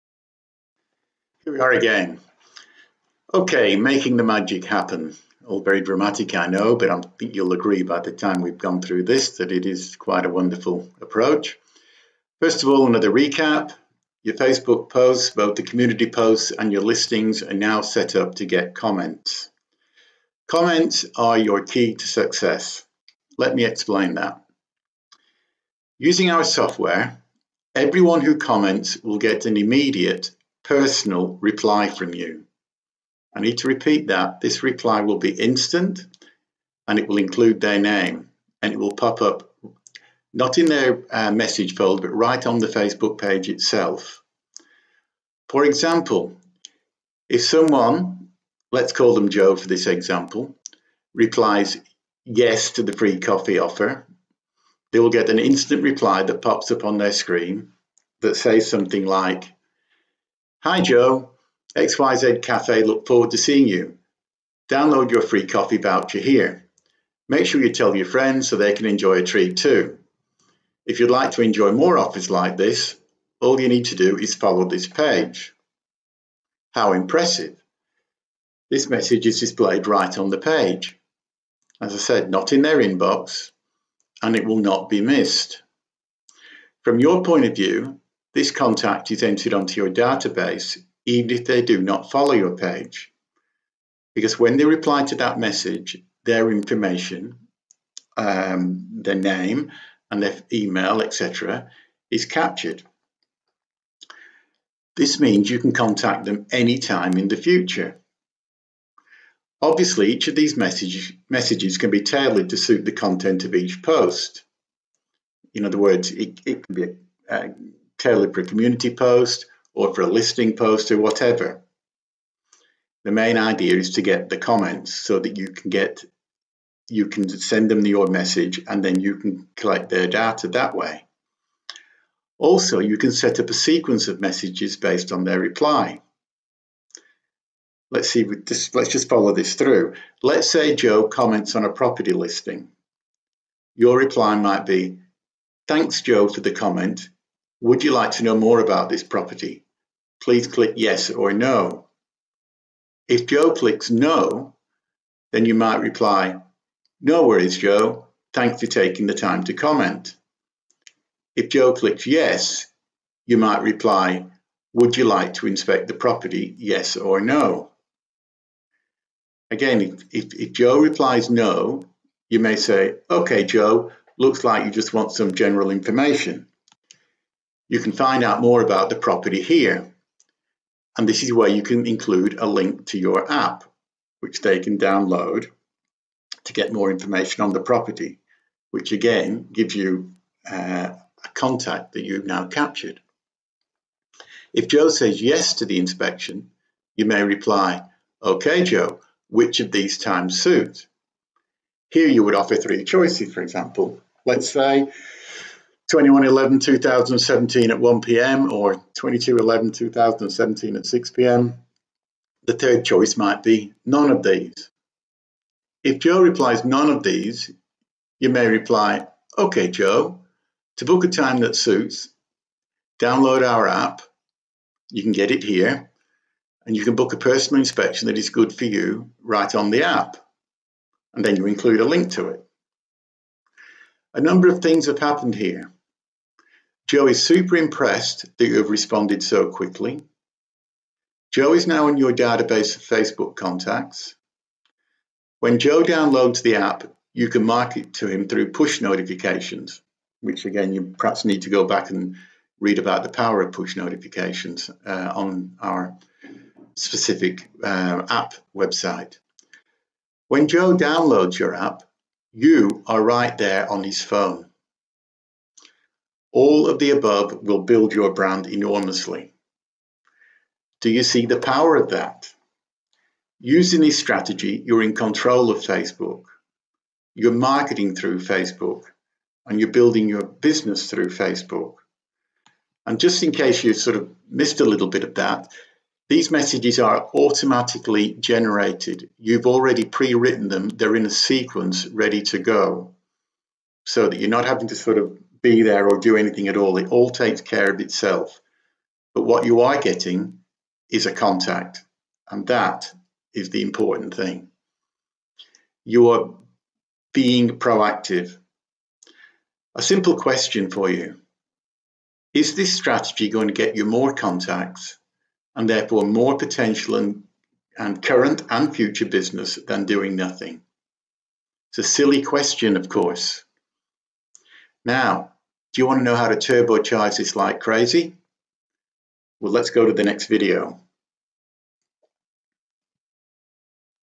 Facebook marketing for real estate agents
Part 6 of a series of videos explaining Facebook marketing strategy for real estate agents.